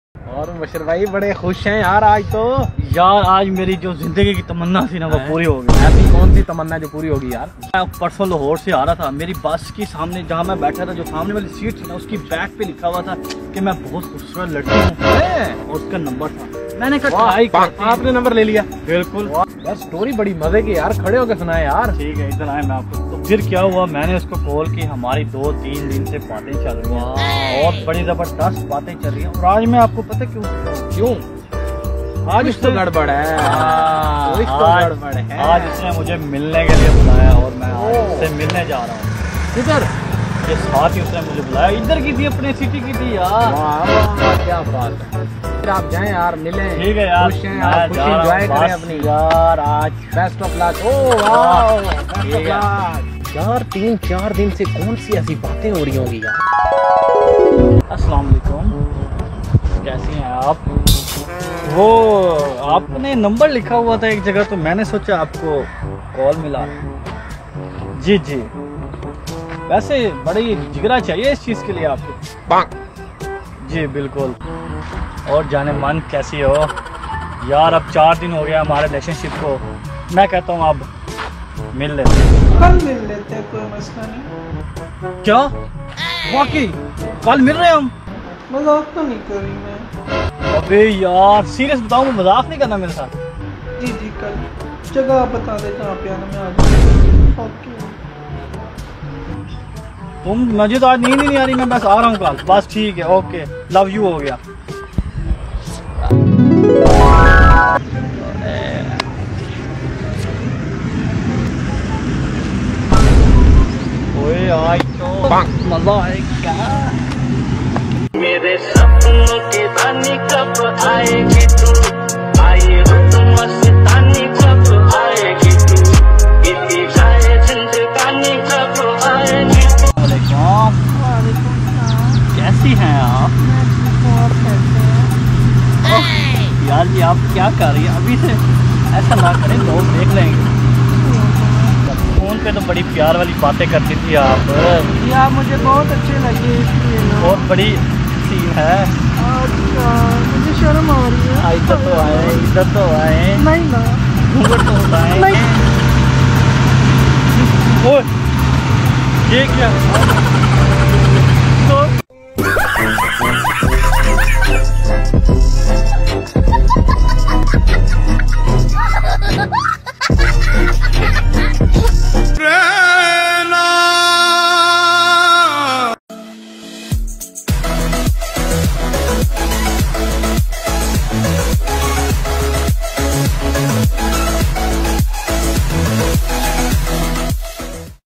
Girl Number Prank Gone Wrong | Funny Prank
In This Video we prank with our friend mubashir.The Girl number prank gone wrong beacuse mubashir beleive she is a girl but when he come to meet number girl that was a boy then mubashir heart broken.